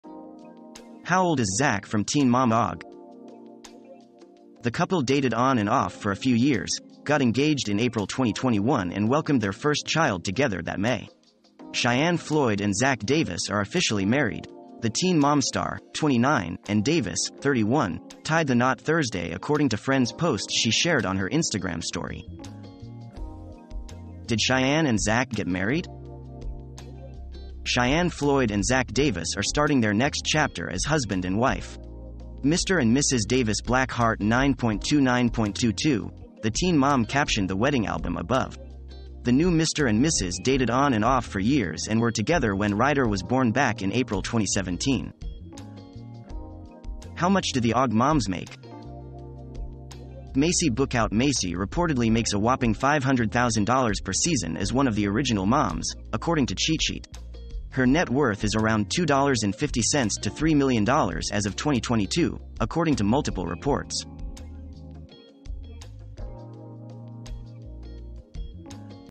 How old is Zach from Teen Mom OG?
Are Cheyenne And Zach Still Together

00:23 - How old is Zach from Teen Mom OG?
00:46 - Did Cheyenne and Zach get Married?
01:09 - How much do the OG moms make?